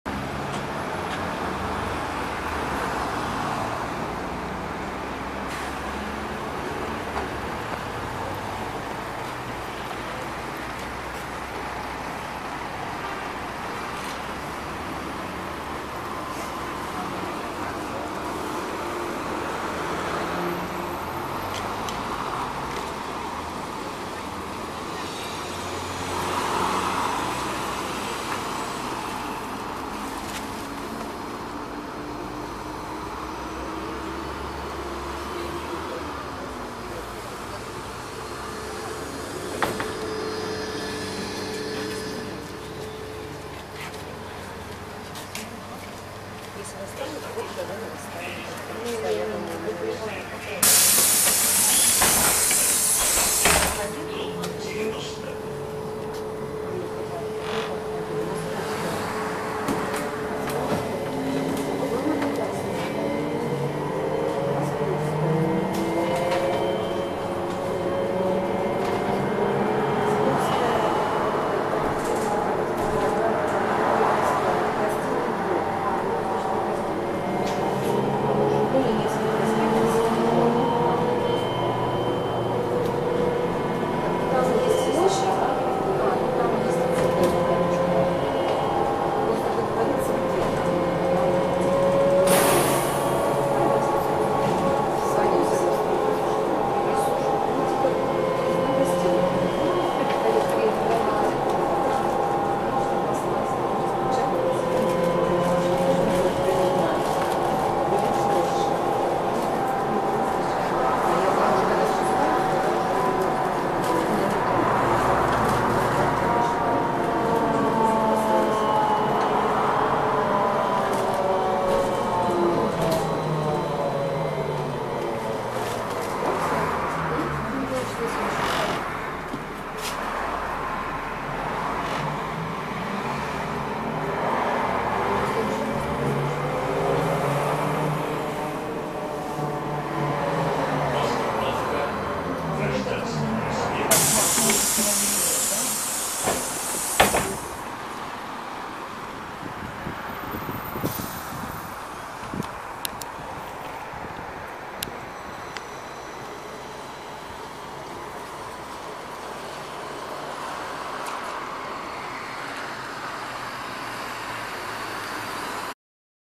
Троллейбус Санкт-Петербурга 8-263: ТролЗа-5265.00 "Мегаполис" б.5201 по №31 (06.07.18)
Троллейбус ТролЗа-5265.00 "Мегаполис", бортовой 5201, следующий по маршруту №31.
Видео снято по: Гражданский проспект (от ост. "ст. м. Академическая"  до ост. "Гражданский пр., 90")
Дата съемки: 6 июля, 2018 в 22:17